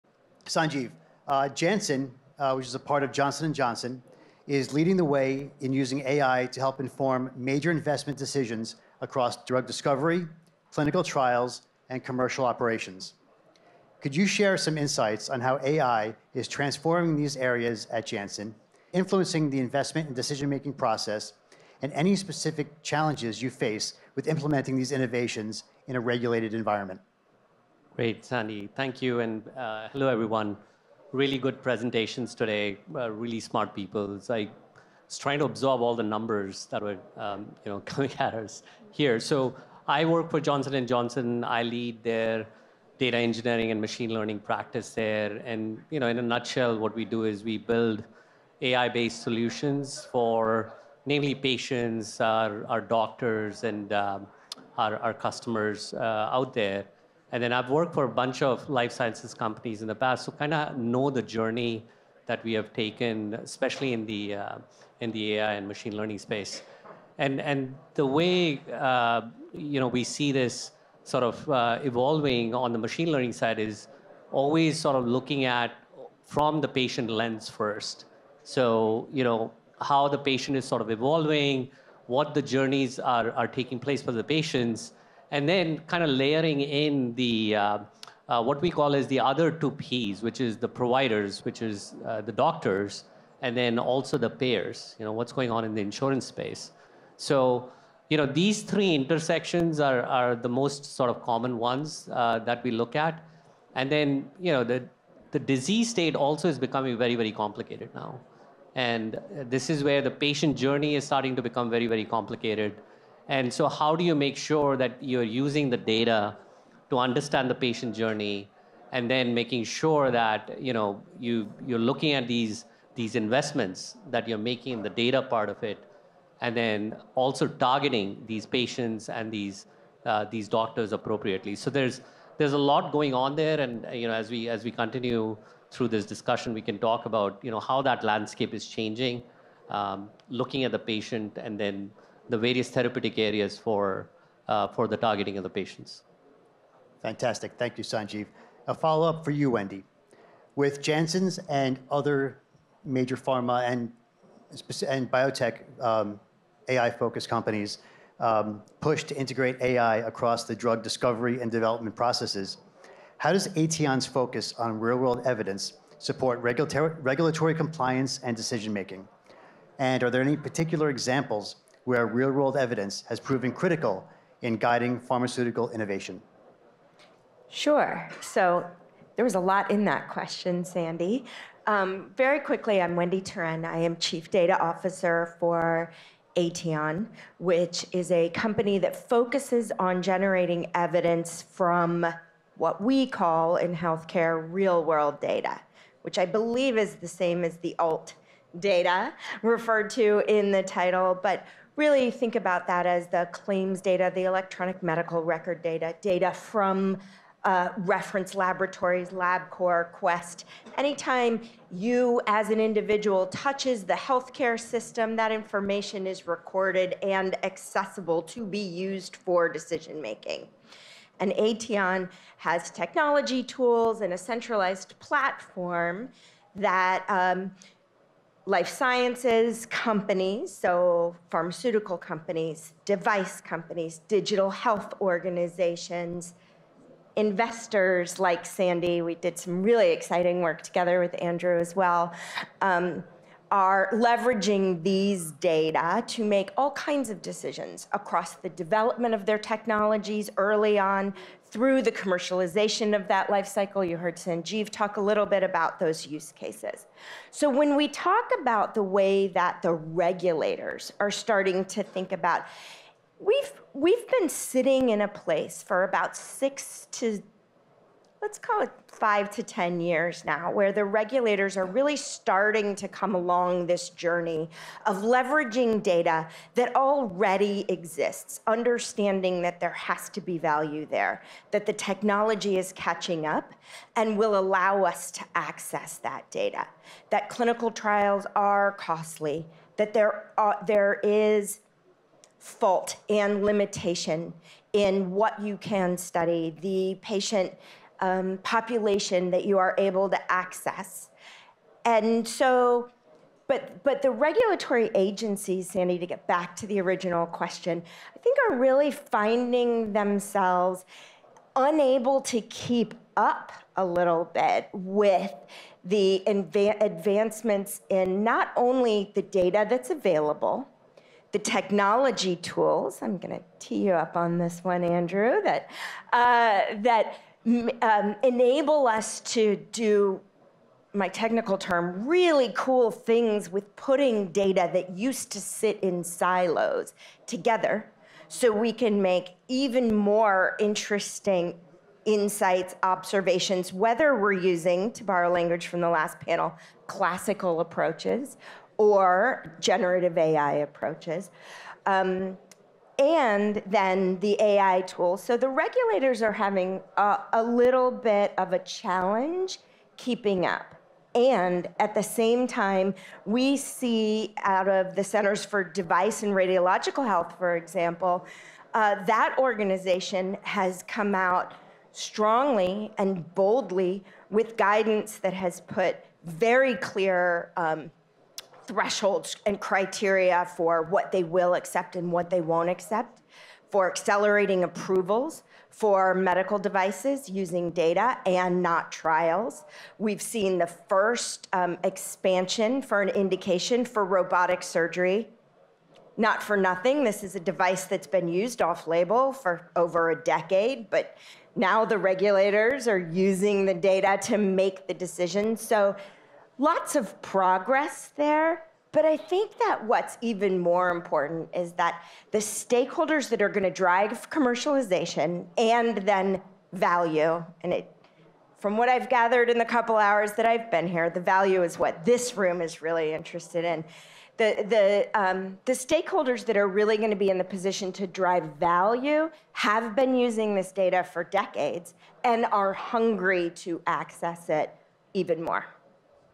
𝐄𝐃𝐔 𝐒𝐞𝐫𝐢𝐞𝐬💡How's AI Transforming Pharma Innovation in Drug Discovery, Clinical Trials & Development?
EDU Series 💡 How is AI Transforming Pharma Innovation in Drug Discovery, Clinical Trials & Development?

PANEL - The Convergence of Tech, Alt Data, AI & ML and other Breakthrough Advancements in Healthcare & Life Sciences: The ongoing transformation creates exciting opportunities for investors and alpha creation. Explore market trends, investment strategies, regulatory hurdles, and future prospects as we head into 2025.

Discover intricacies of breakthrough advancements in healthcare and life sciences, its future, and more. Get the answers from our expert panelists featuring:

Moderator
Sandy Balkin, Ph.D. SVP Strategy and Analytics, Royalty Pharma

Speakers:
Andrew Goldberg, COO, HealthVerity
Wendy Turenne, COO, Aetion
Julia Fitzgerald, Product Management Director, Earnest Analytics
Sanjiv Koshal, Head of Machine Learning, Johnson & Johnson